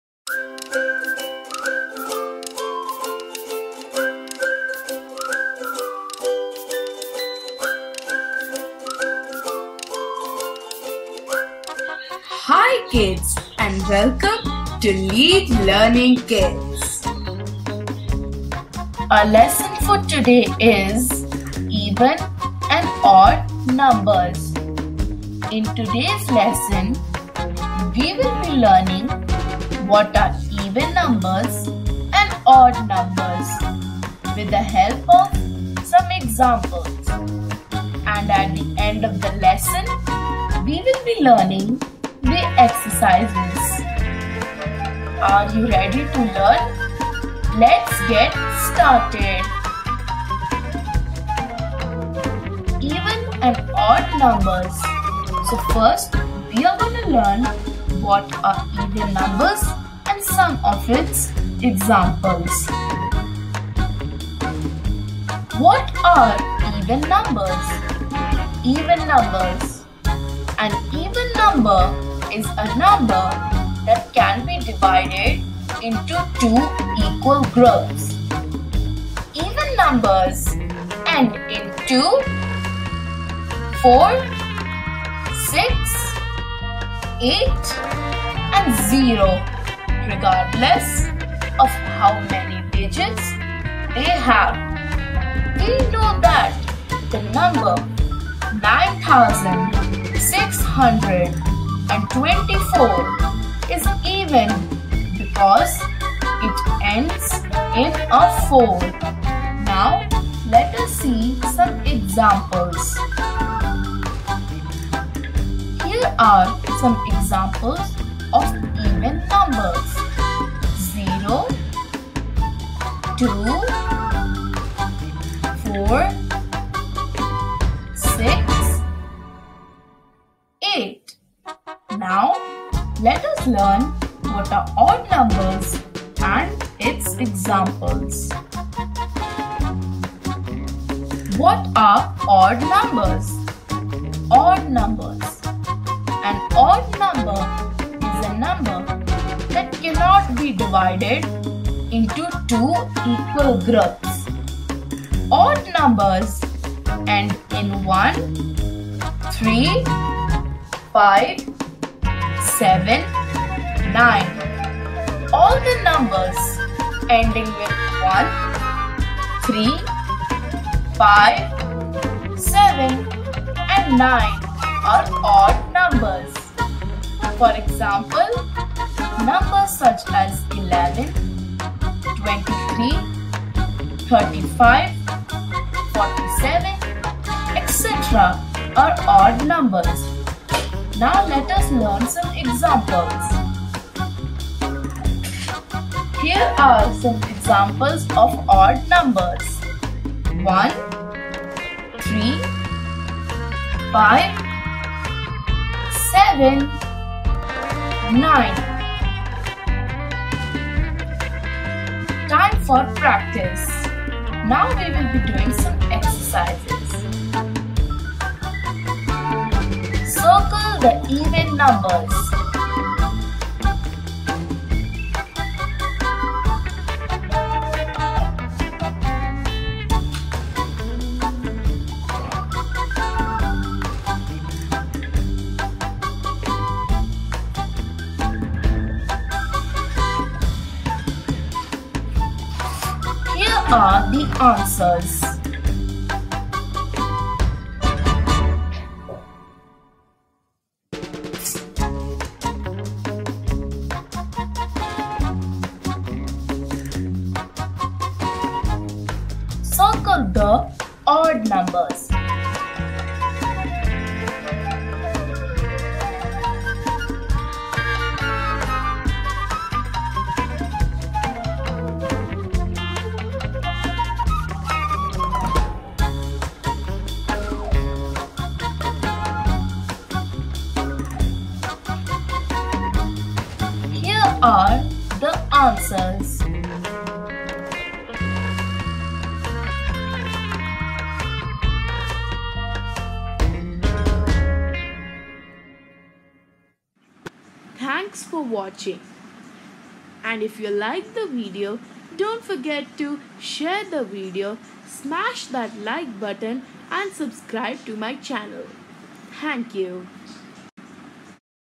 Even And Odd Numbers|for kids|Lead Learning Kids
This vid is about even and odd numbers. very useful for kids.easy to understand.